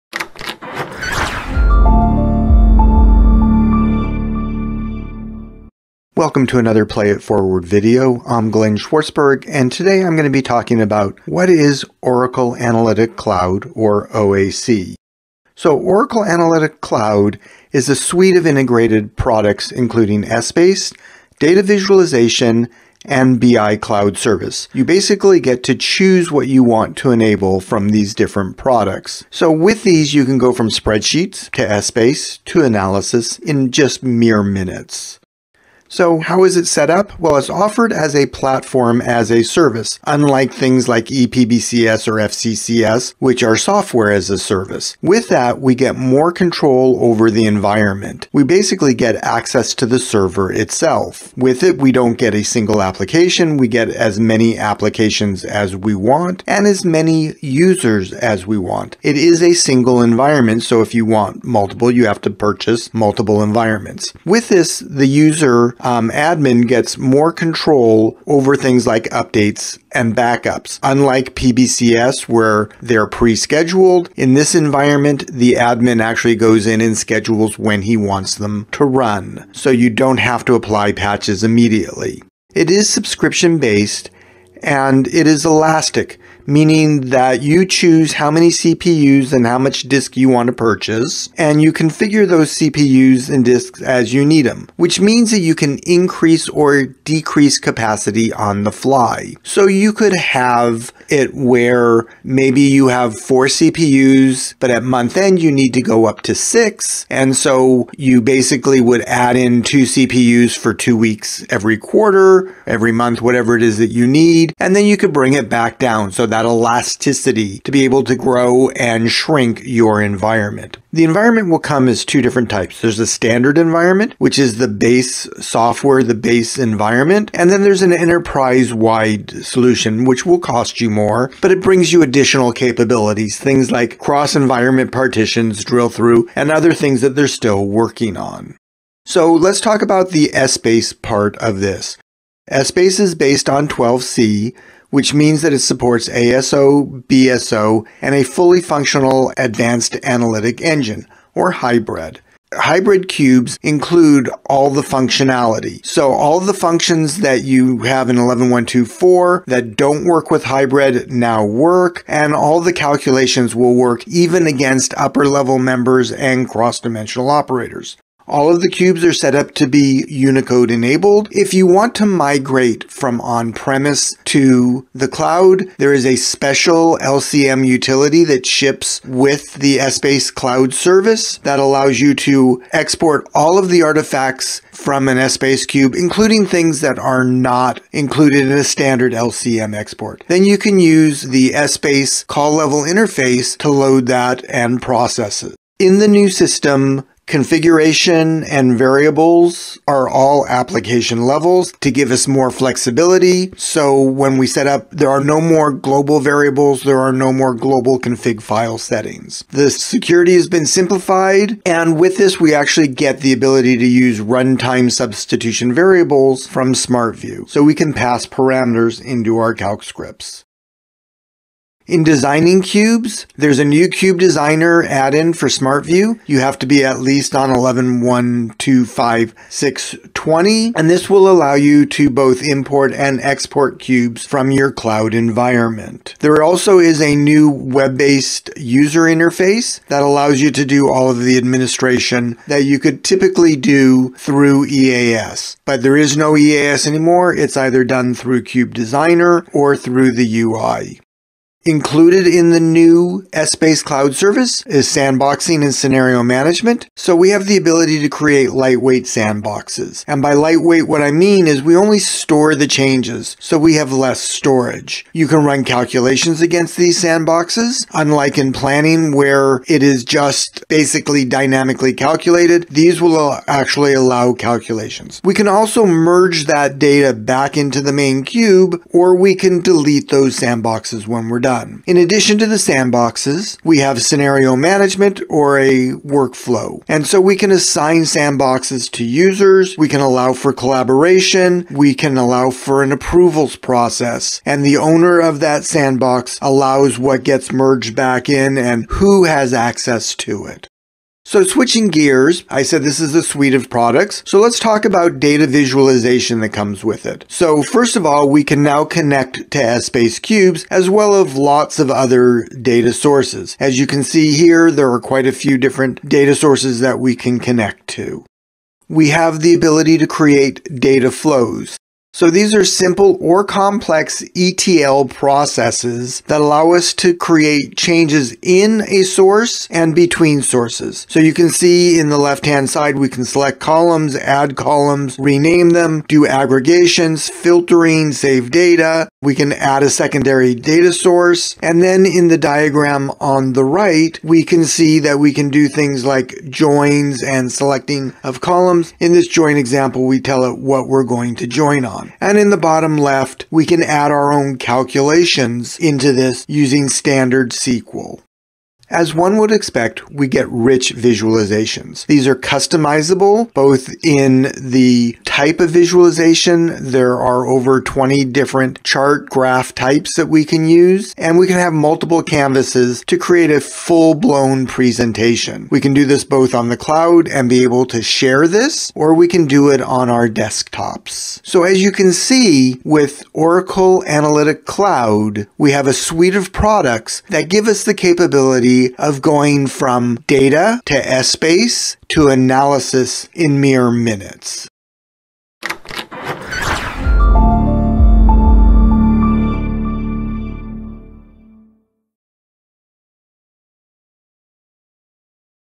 What is Oracle Analytics Cloud (OAC)?
interRel presents Look Smarter Than You Are With Oracle Analytics Cloud: What is OAC presented by Glenn Schwartzberg.

Oracle Analytics Cloud (OAC) delivers business analytics for traditional data and big data across the entire enterprise.  Built on proven Oracle technologies and cloud infrastructure, OAC is a comprehensive product portfolio built for the Cloud. It covers a wide variety of needs across business intelligence, big data analytics, and embedded SaaS analytics.  With Essbase as its key technology, Oracle Analytics Cloud (OAC) merges all of Oracle’s Business Intelligence products into one unified solution.